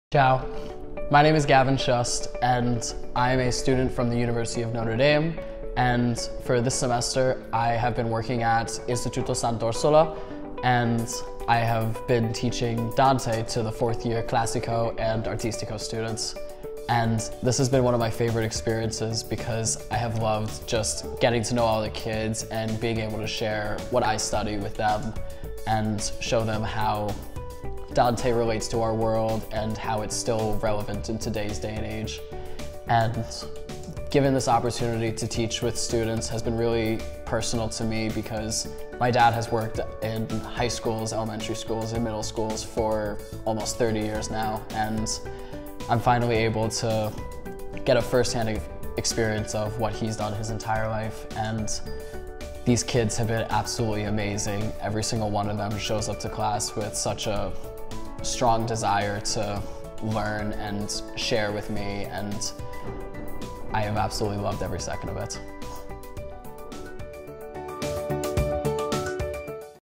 Gavin, studente dell’Università di Notre Dame in Indiana, racconta la sua esperienza di co-teaching
I nostri Licei hanno ripreso, dopo due anni di pandemia, lo scambio con la Notre Dame University attraverso un progetto di co-teaching con Gavin Shust, studente di Lingua e Letteratura italiana e Scienze Politiche.

Il progetto prevede 10 ore settimanali di approfondimento sulla letteratura italiana e sulla filosofia politica in lingua straniera.

Le lezioni con Gavin sono per i ragazzi un'occasione unica di conoscere il metodo didattico e di lavoro di un'università americana e potenziare la conoscenza della lingua inglese.